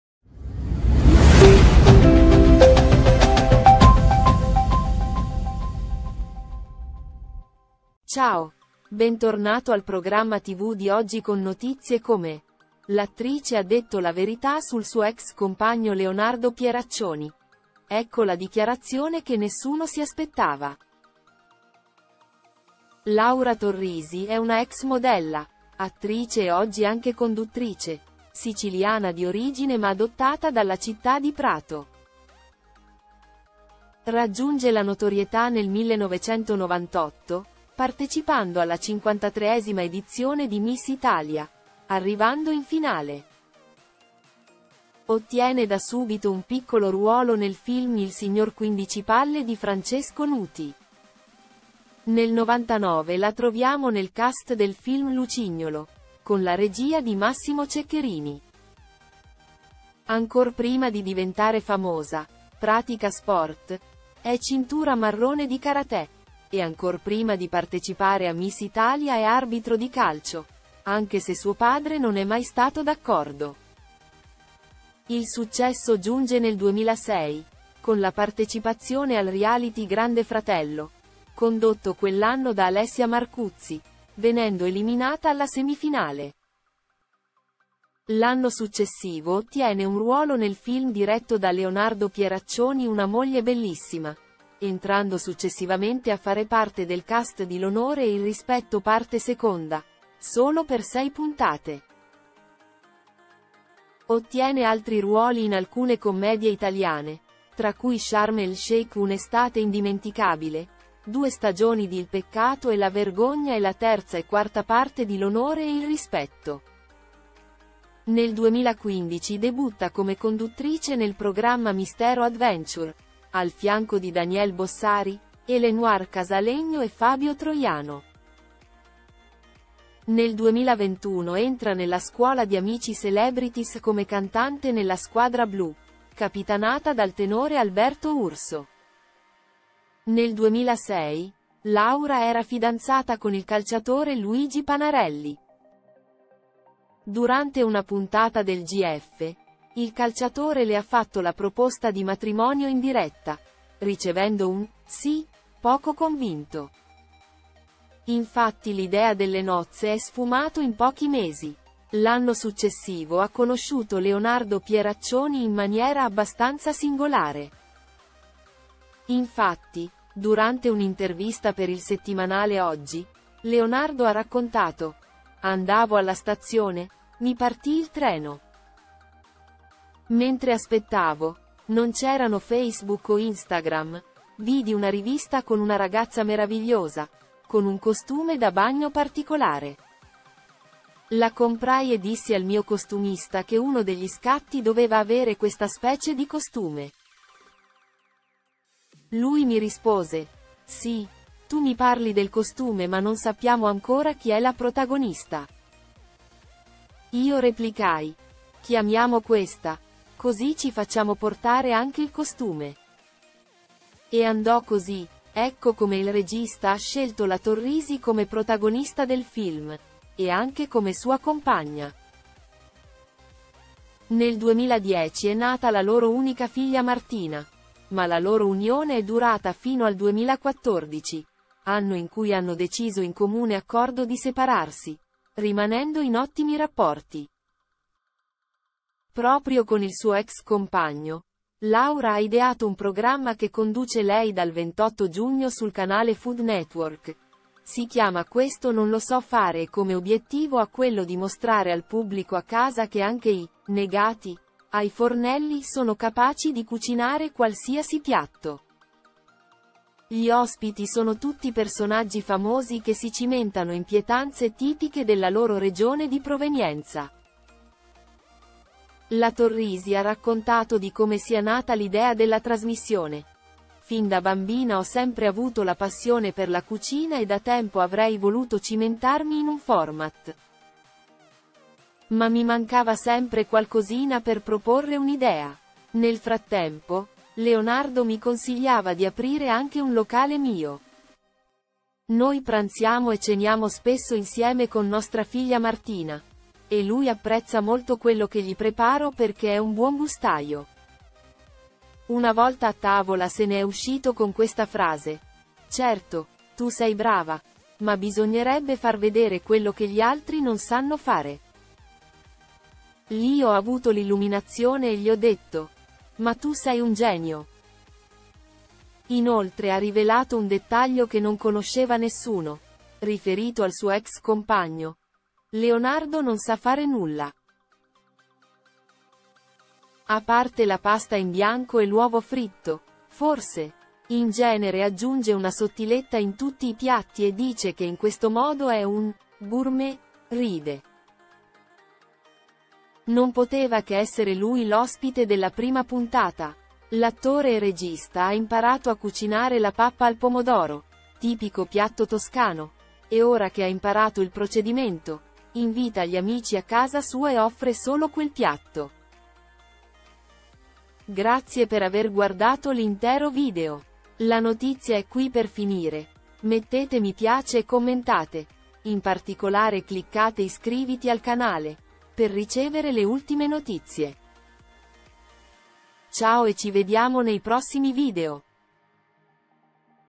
Laura Torrisi, la stoccata a Pieraccioni è clamorosa “Non sa fare nulla”
L’attrice ha detto la verità sul suo ex compagno Leonardo Pieraccioni: ecco la dichiarazione che nessuno si aspettava.

Laura Torrisi è una ex modella, attrice e oggi anche conduttrice, siciliana di origine ma adottata dalla città di Prato. Raggiunge la notorietà nel 1998, partecipando alla cinquantatreesima edizione di Miss Italia, arrivando in finale. Ottiene da subito un piccolo ruolo nel film Il signor Quindicipalle di Francesco Nuti. Nel ’99 la troviamo nel cast del film Lucignolo, con la regia di Massimo Ceccherini.

Ancor prima di diventare famosa, pratica sport: è cintura marrone di karate, e ancor prima di partecipare a Miss Italia è arbitro di calcio, anche se suo padre non è mai stato d’accordo. Il successo giunge nel 2006, con la partecipazione al reality Grande Fratello, condotto quell’anno da Alessia Marcuzzi, venendo eliminata alla semifinale.